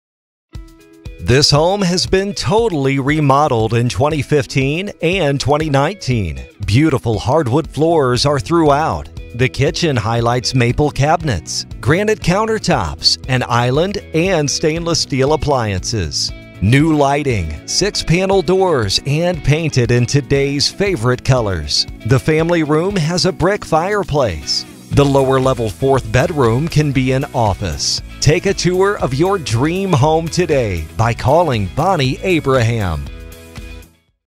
1073 Harvard Lane Buffalo Grove, IL | ColdwellBankerHomes.com
1073 Harvard Lane, Buffalo Grove, IL

Totally remodeled 2015 and 2019. Beautiful hardwood floors throughout. Kitchen with maple cabinets, granite countertops, island and stainless steel appliances. New lighting, six panel doors and painted in todays favorite colors! New bathrooms! Family room with brick fireplace! Lower level fourth be